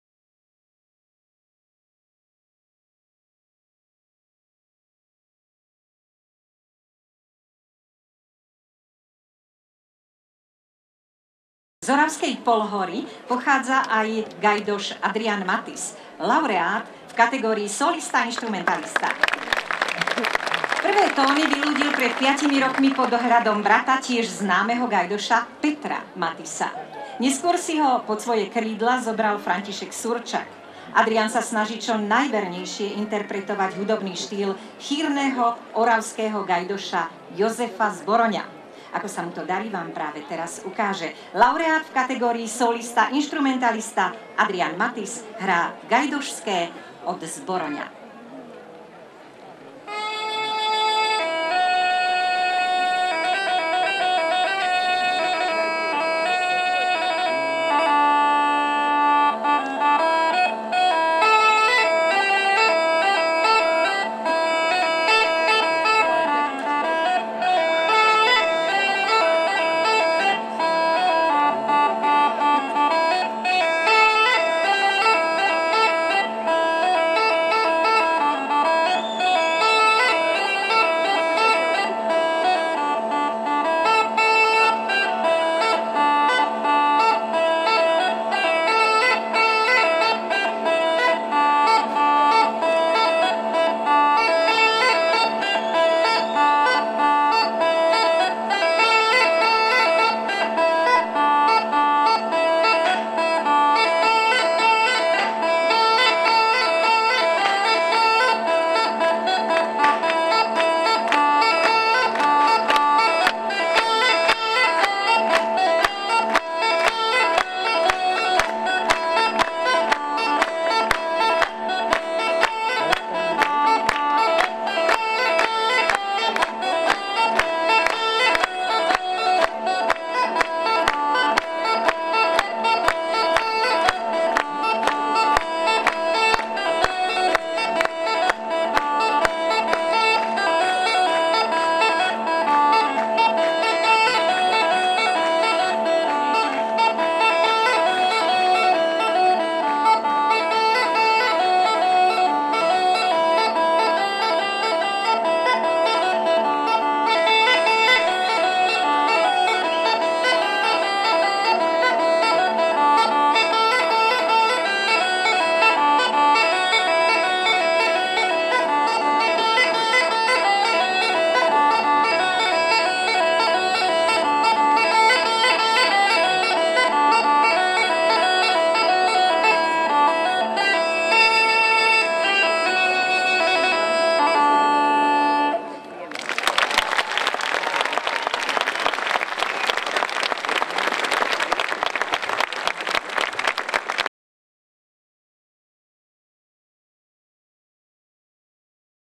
20160702 62 VYCHODNA MALI MUZIKANTI A SPEVACI ADRIAN MATIS Z ORAVSKEJ POLHORY
62 FOLKLORE FESTIVAL VYCHODNA
MALI MUZIKANTI A SPEVACI ADRIAN MATIS Z ORAVSKEJ POLHORY